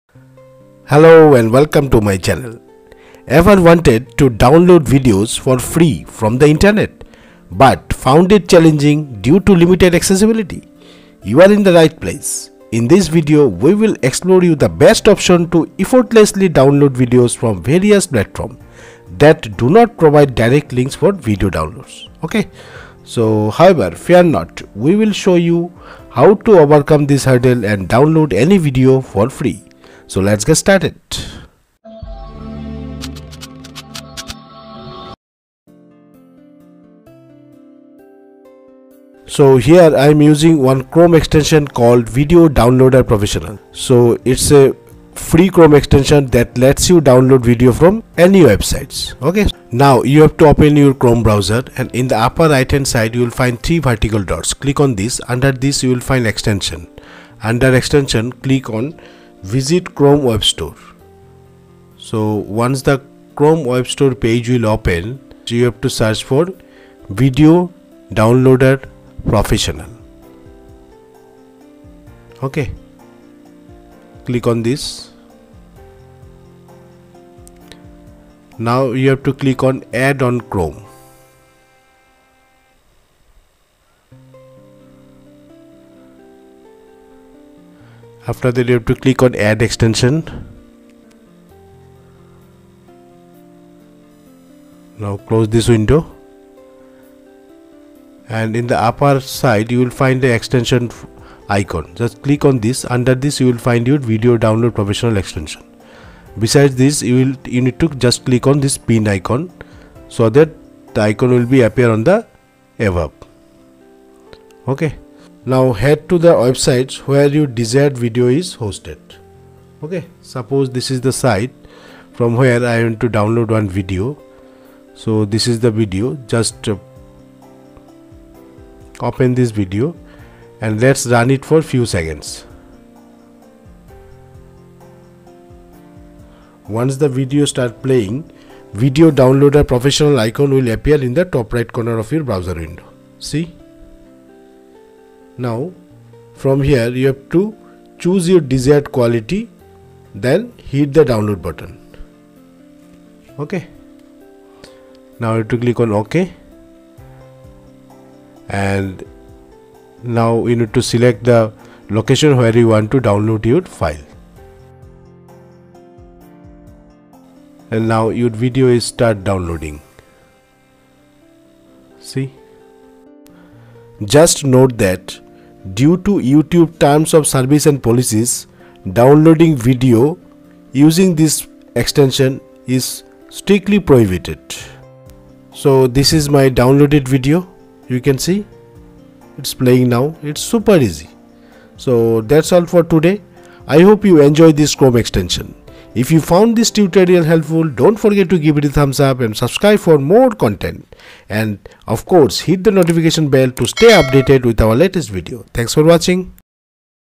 Download ANY Video From ANY Website (This Chrome Extension is a Game Changer!)
Download ANY Video From ANY Website with Vsave Quickly Save Videos From 1,000+ Sites Without Any Hassle!

Best method to download, and convert any media from dozens of platforms, including YouTube and social media. Simple, free, and safe

🎥 Unlock the world of video downloads with our latest tutorial! In this comprehensive guide, we explore the power of Chrome extensions, revealing how you can download videos from various websites hassle-free. From Twitter and Facebook to Vimeo and more, we've got you covered! 🌐 However, please be aware that YouTube videos are exempt from this extension due to copyright policies. Always respect content creators' rights. Ready to master the art of video downloads? Watch now and become a pro! 🔗
This is a Complete Step by Step Tutorial